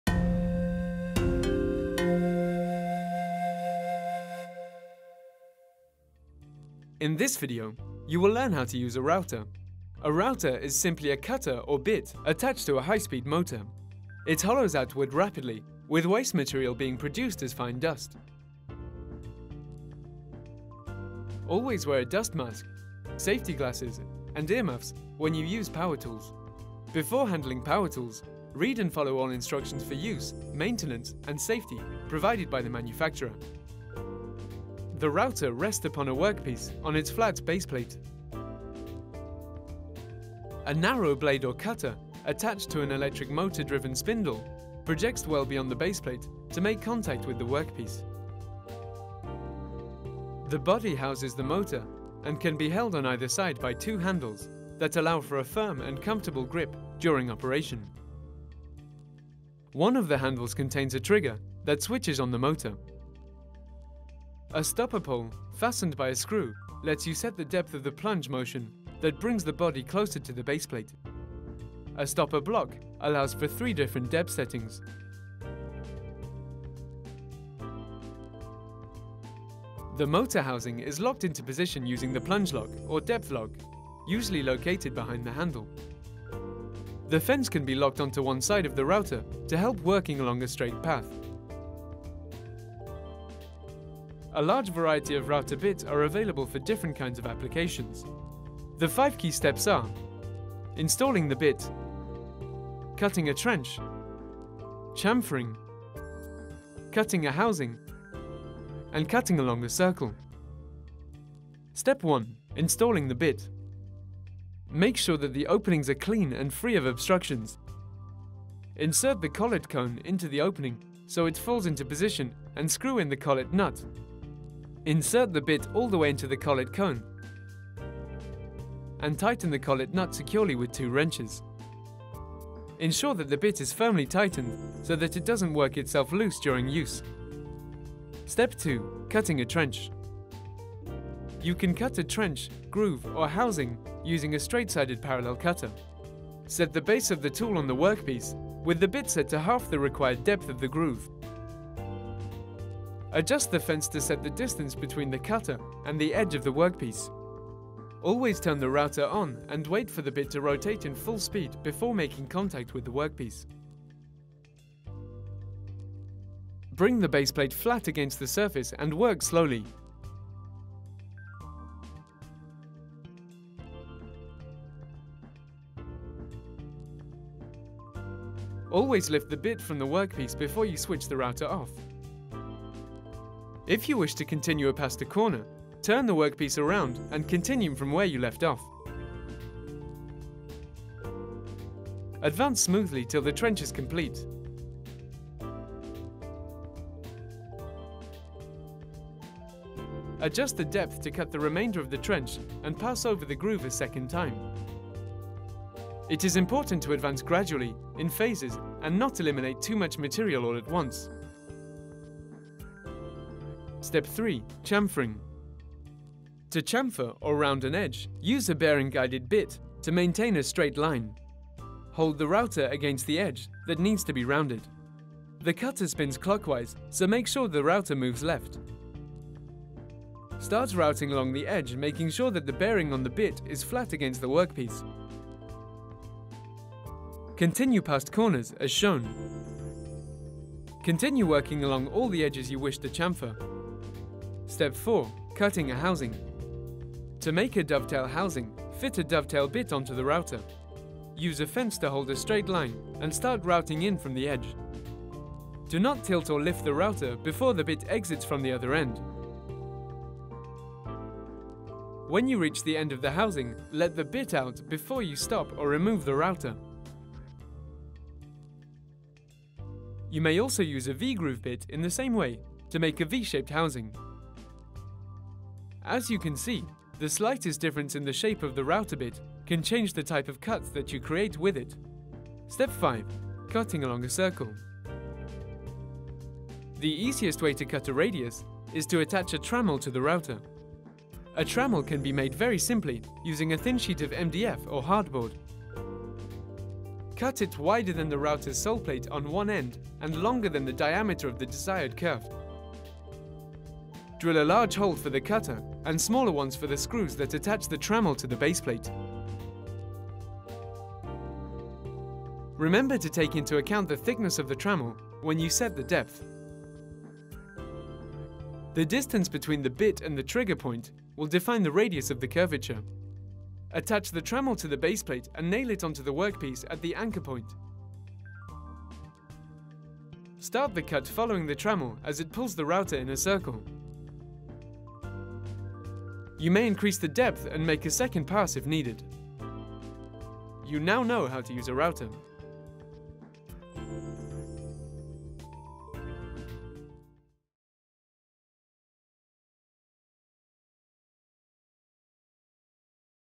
How to Use a Router | Woodworking
This video will instruct you on how to use a router.

This content will serve as an aid for vocational development.
To learn how to use a planer, watch the next video in this series.

Using a router for the first time? Help is at hand! Watch this short tutorial video to learn how to use a router safely and effectively in your woodworking and carpentry projects.

The Woodworking Program was created thanks to the support of Kingfisher.